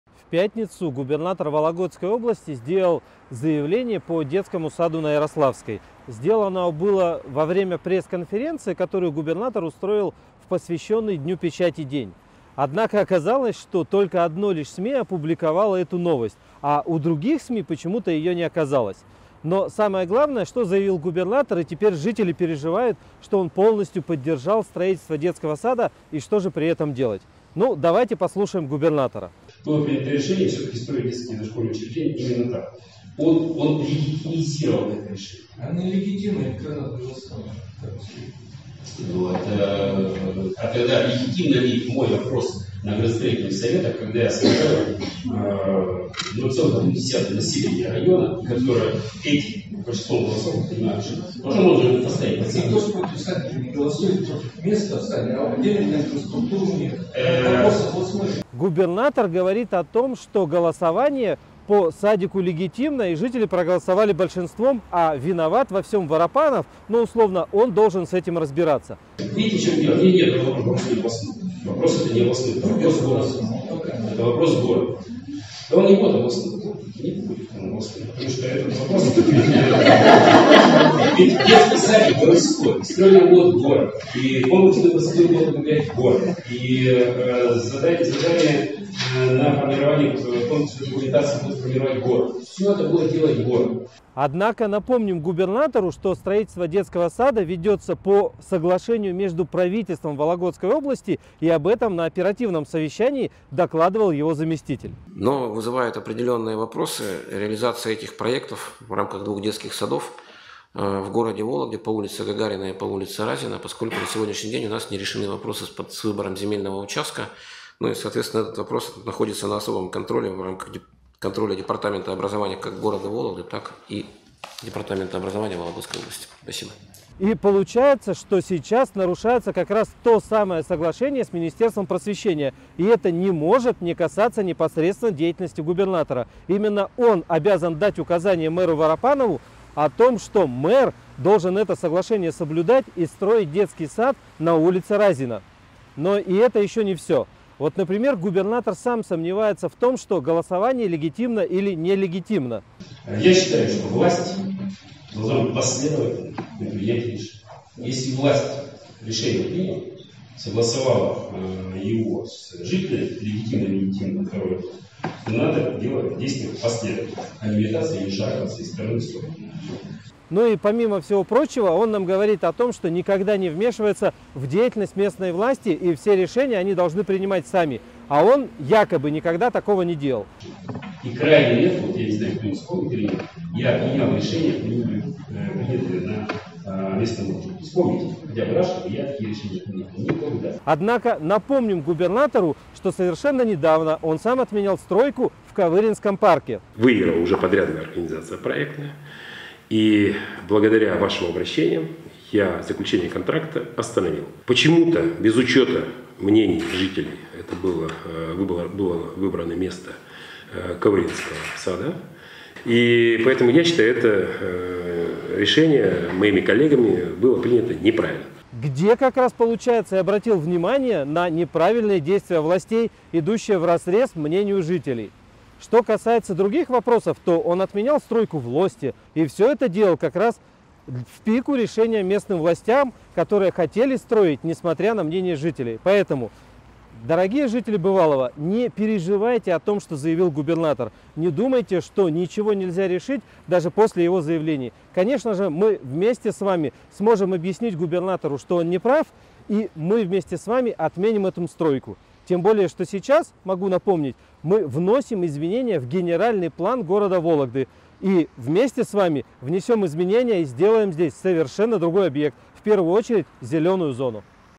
Не переживайте, губернатор умеет исправлять свои и чужие ошибки
В пятницу губернатор Кувшинников провёл пресс-конференцию и пообщался с журналистами в рамках празднования дня печати. Правда оказалось, что день печати немного ограниченный, ведь о пресс-конференции написали только на НьюсВо. Интересно, получили хоть одну благодарность или награду от губернатора? Впрочем, я не об этом. Благодаря журналистам НьюсВо я получил десяток звонков и сообщений в выходные. Ведь они написали, что губернатор заявил о бесповоротном решении строительства детского сада. Ну и, конечно же, люди опять запереживали. Вот я и хочу всем сказать — Не переживайте, губернатор умеет исправлять свои и чужие ошибки.

Причём, несмотря на то, что он отрицает вмешательство в деятельность местной власти и отмену её решений, мы с ходу можем вспомнить пару случаев. Например контракт застройку в Лосте. Или, совсем недавно, застройку в Ковыринском парке.

Поэтому, повторюсь, не переживайте. Лучше собирайтесь 26 января на встречу по обсуждению Генплана, которая пройдёт на Беляева 9 в ДК Льнокомбината с 10 до 12.30.
Там мы продолжим борьбу за сквер на Ярославской.

Поддерживайте ролики, расследования, эфиры и развитие этого канала!